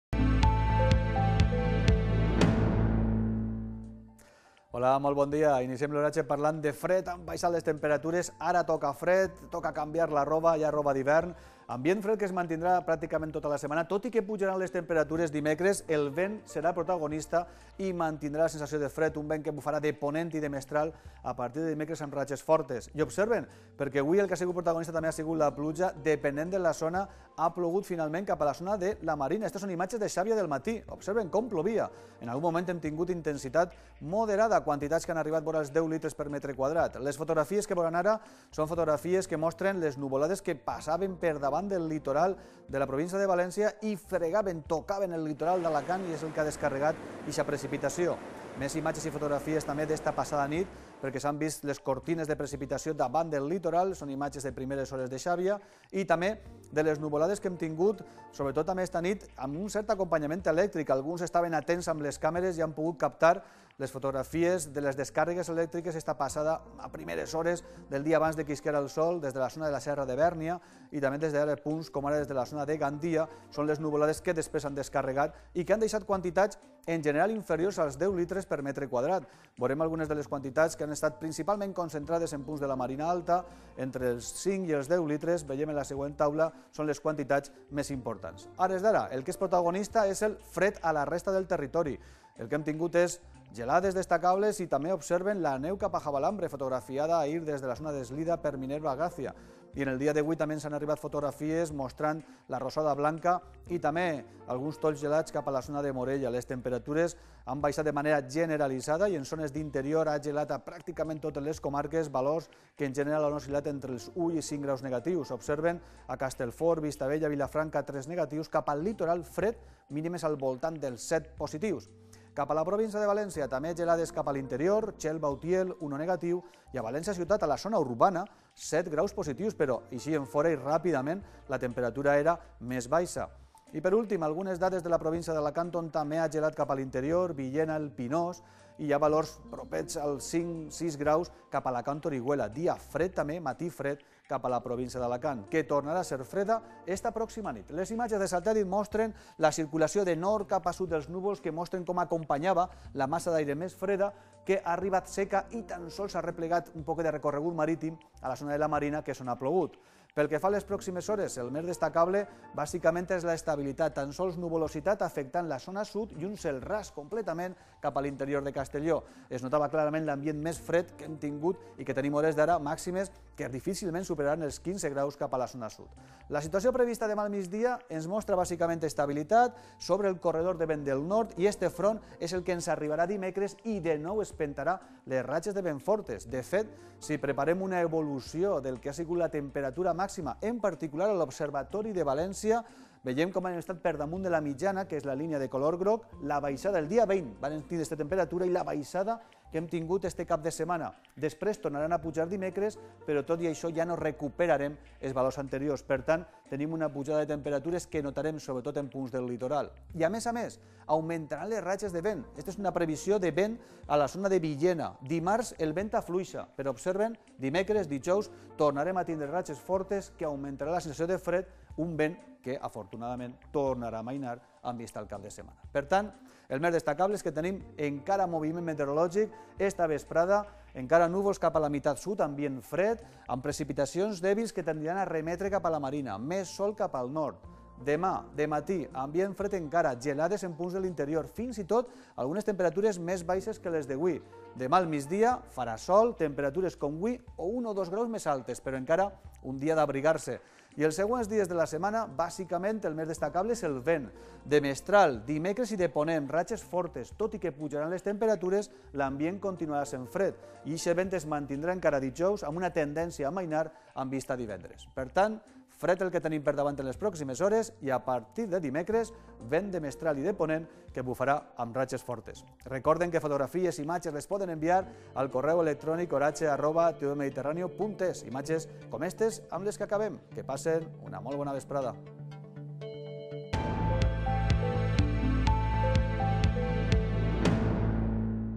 oratge migdia oratgecv 23 novembre 2015
Els dies més freds d'esta setmana dilluns i dimarts.
A partir de dimecres vent fort de ponent  i de mestral.